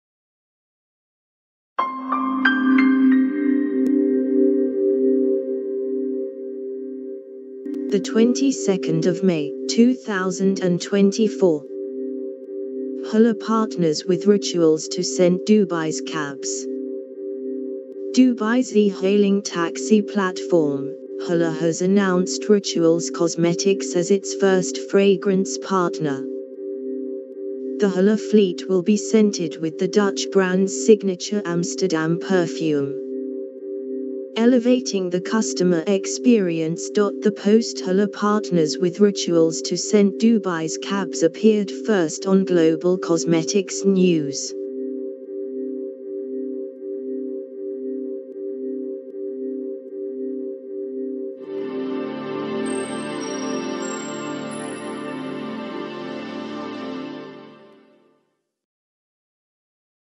[Latest]Hala partners with Rituals to scent Dubai’s cabs
[Contents]
Dubai’s e-hailing taxi platform, Hala has announced Rituals Cosmetics as its first fragrance partner; the Hala fleet will be scented with the Dutch brand’s signature Amsterdam perfume, elevating the customer experience.The post Hala partners with Rituals to scent Dubai’s cabs appeared first on Global Cosmetics News.

[May, 2024]

[Cosmetics]

[Global]